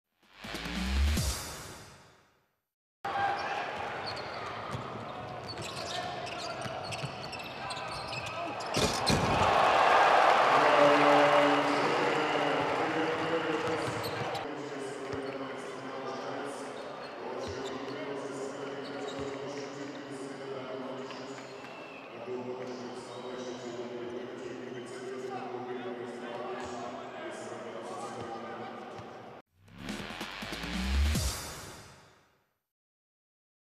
Grant Jerrett with a PUTBACK! (Igokea - Cedevita, 2.2.2019)
Grant Alexander Jerrett is proving his added value for Igokea. That's how he corrected the alley-oop attempt by Filip Adamović!